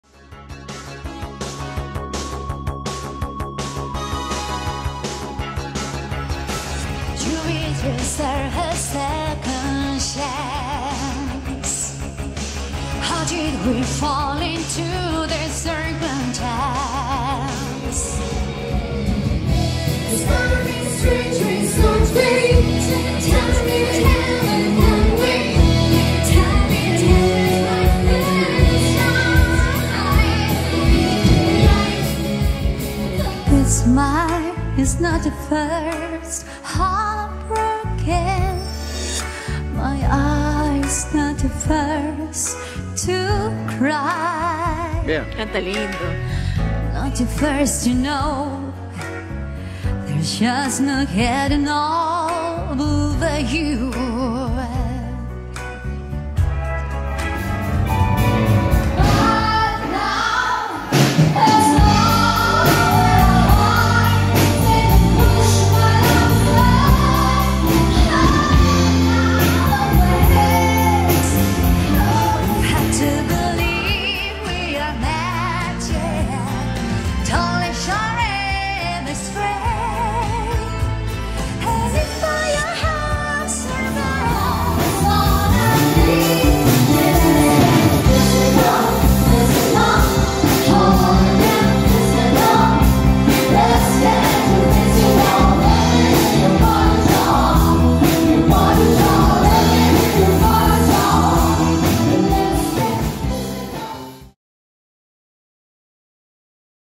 Video promoción 2023 Olivia Experience Banda tributo a Olivia Newton John
Muchas gracias a todos los que nos siguen este es un pequeño video promo que tenemos de nuestra banda tributo, que se ha presentado exitosamente en varios lugares, encuentranos